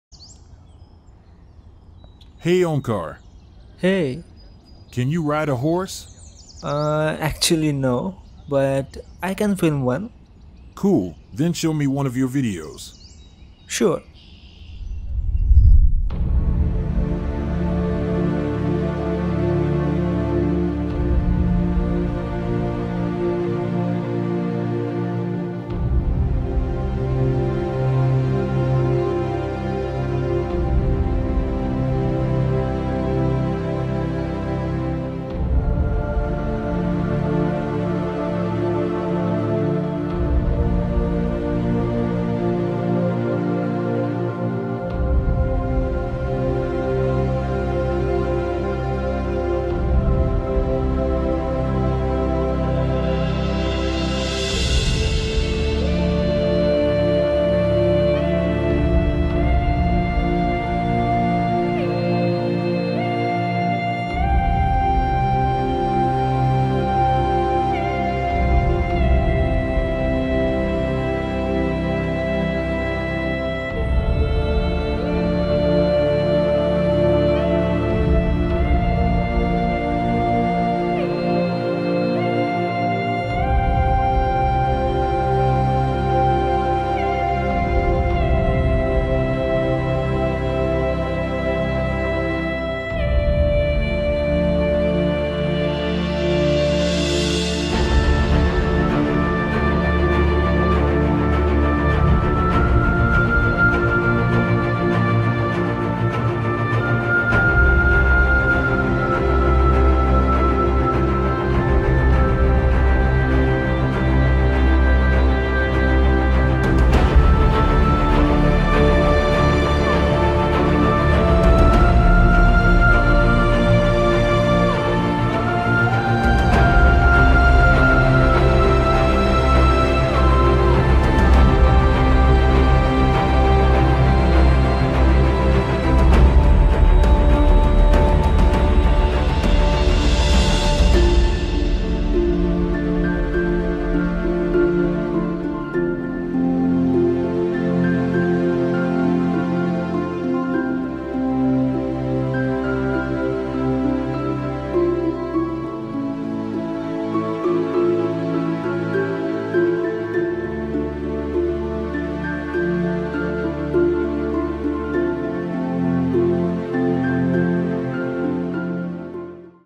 Trails Of Sahyadri | A Cinematic Journey on Horseback - By Omkar Pendsay
Embark on a breathtaking cinematic journey as we delve into the mesmerizing world of horse trails. Join us on an unforgettable adventure as we traverse lush landscapes, rugged terrains, and enchanting forests. With state-of-the-art camera equipment and an eye for capturing the essence of nature, we bring you a visually stunning experience that transports you right into the heart of the horse trail. Witness the graceful gallops, the magnificent landscapes, and the profound connection between horse and rider. This cinematic masterpiece is a testament to the beauty and harmony that exists between humans and the majestic creatures that roam these trails. Join us on this immersive cinematic exploration today!